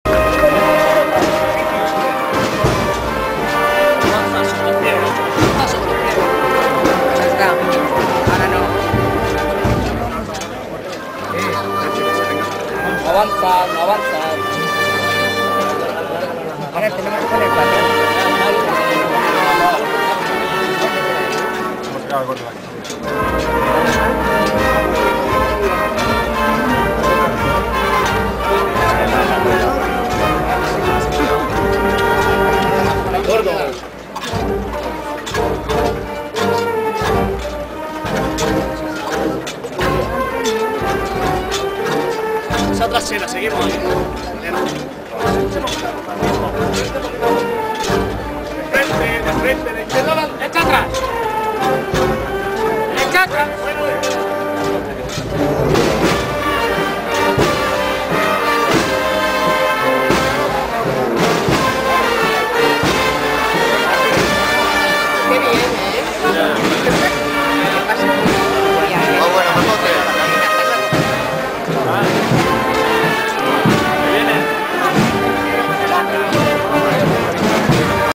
PERCHELERA.VIERNES DOLORES 2008.CIUDAD REAL X
Virgen de los Dolores haciendo la maniobra de salida del convento de las Hermanas de la Cruz de Ciudad Real la noche del viernes de dolores de 2008